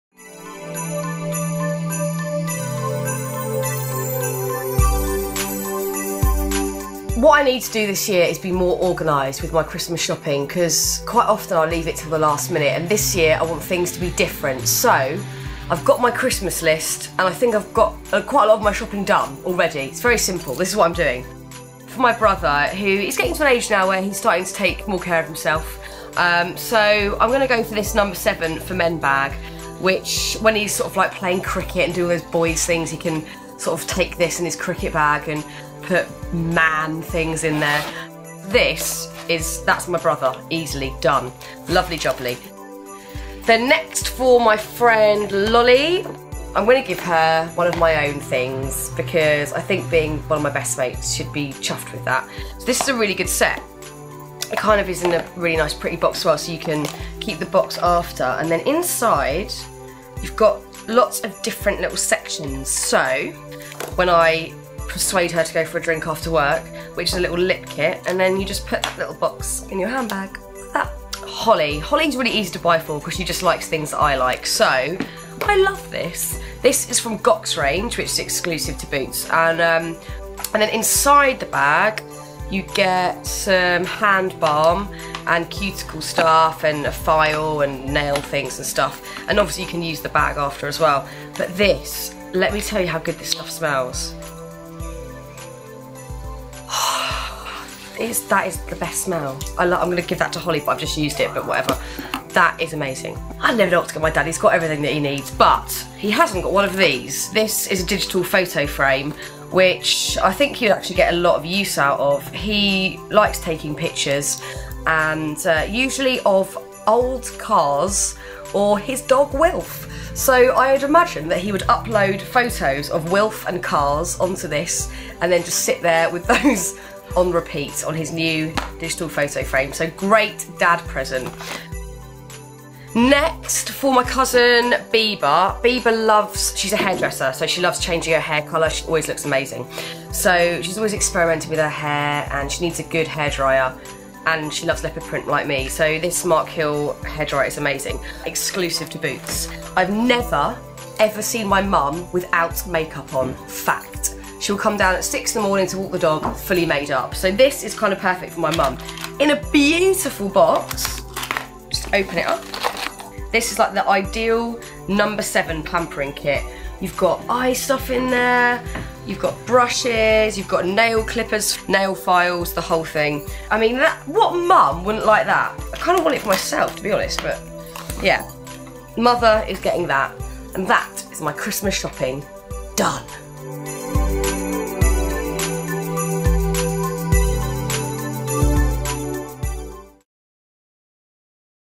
Fearnes Christmas shopping list
Exo design produced two promotional videos with Fearne Cotton promoting her Christmas range.